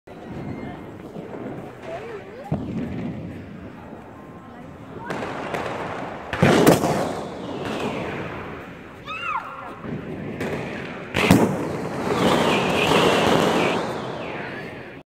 3 inch green shell (Diwali fireworks) |Guwahati, Assam
A brandless shell with a green break🎆